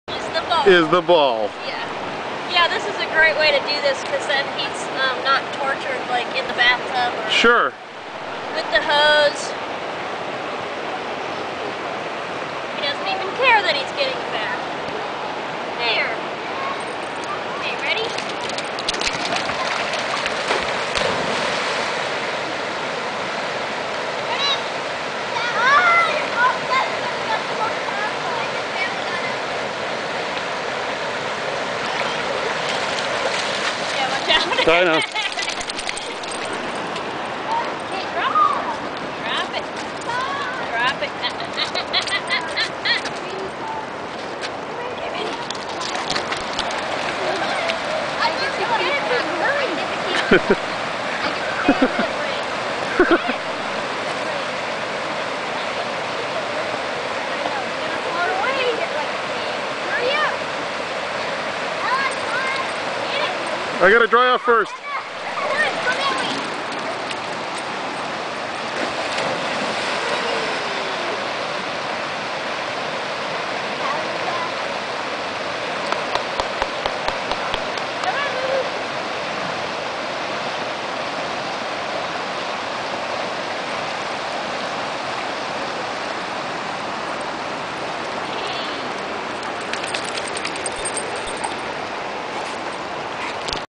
Boo Gets a Bath
At Riverside Campground, a "staff" dog gets a bath, but never really notices...